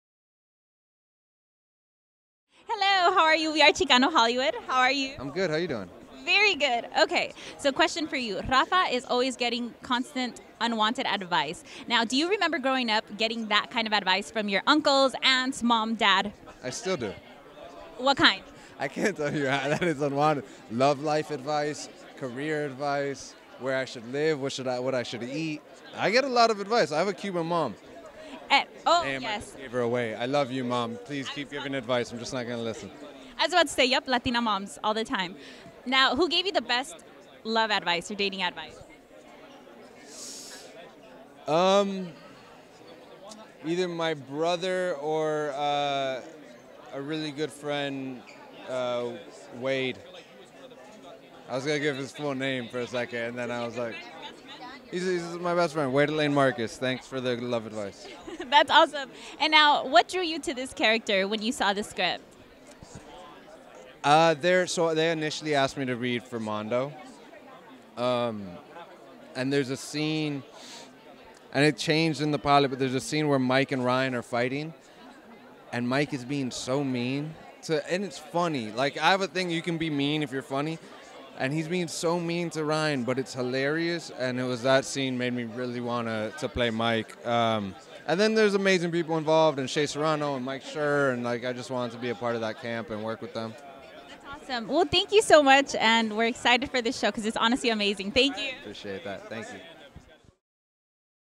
Henri Esteve reveals auditioning for different characters in the series "Primo"
Henri Esteve plays Mike in the series “Primo” where he plays one of 5 uncles who play a big role in Rafa’s life. He says initially he was meant to play Mando, but when he saw the scene of Mike being mean to Ryan that made him want to play Mike instead.